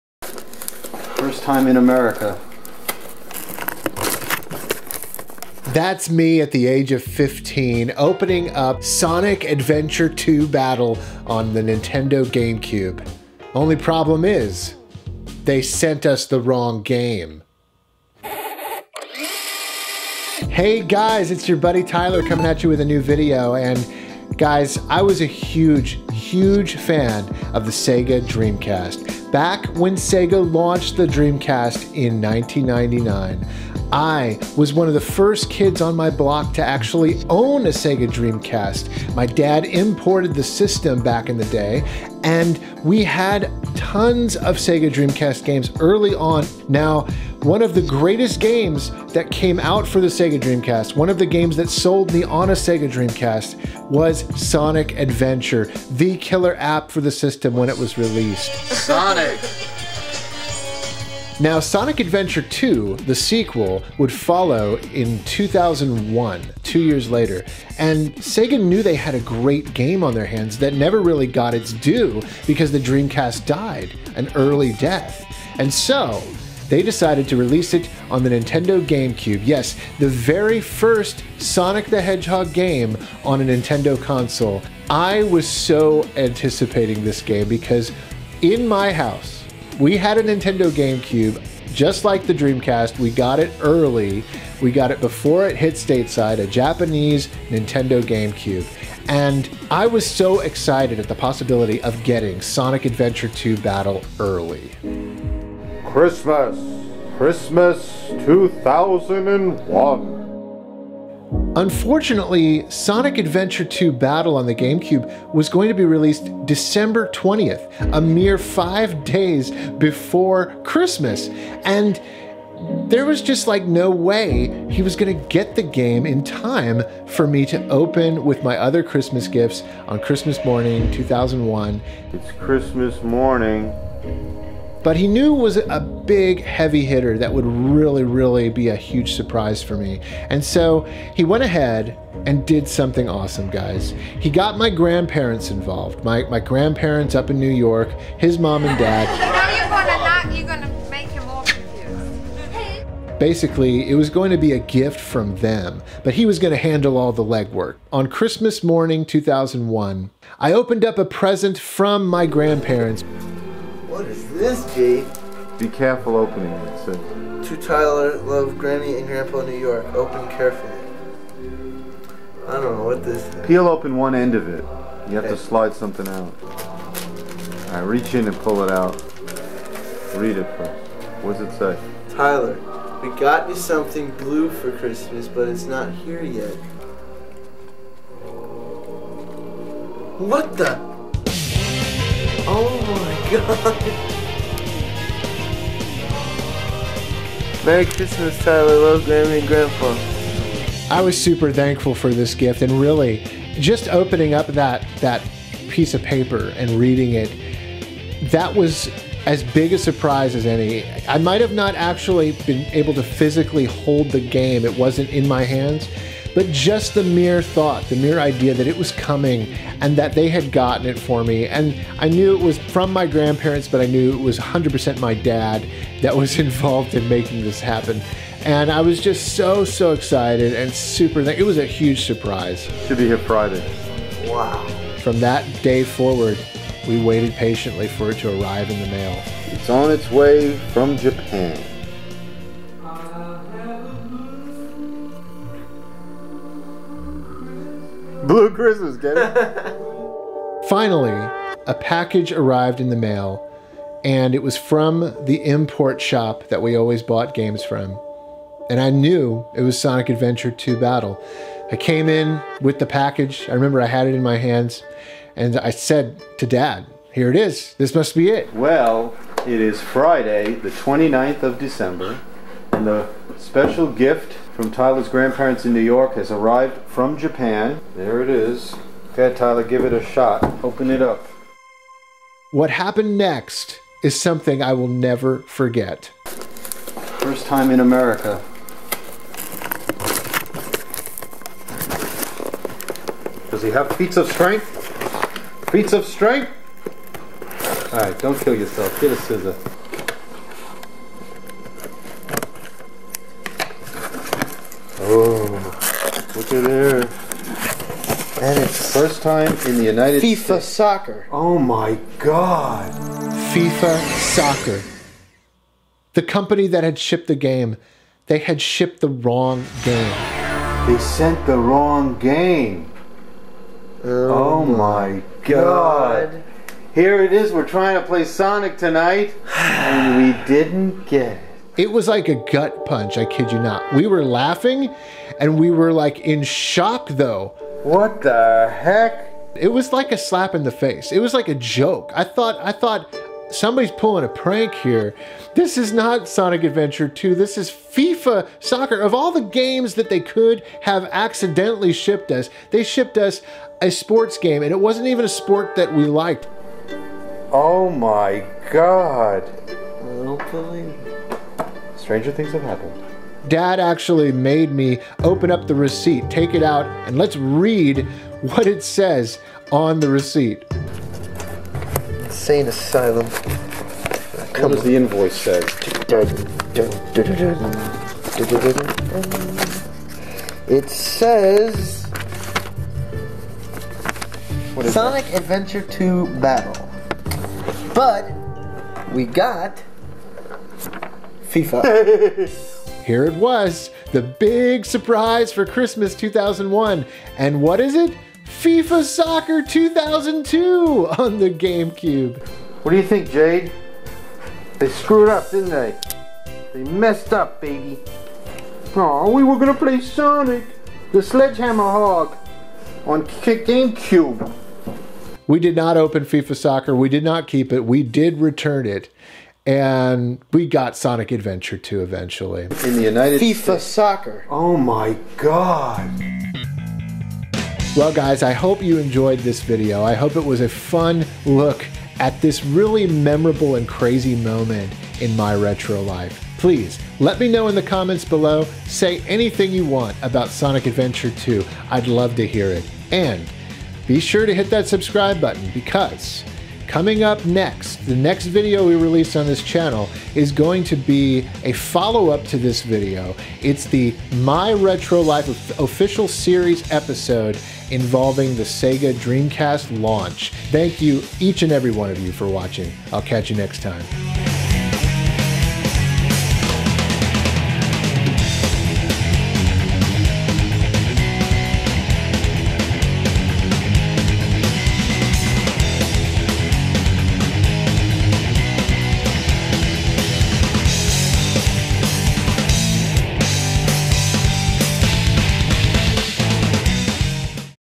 The Sonic Adventure 2 Battle Christmas SCREW UP of 2001 - My Retro Life
During Christmas of 2001, I expected Sonic Adventure 2 Battle on Gamecube - but what I got was a lump of coal.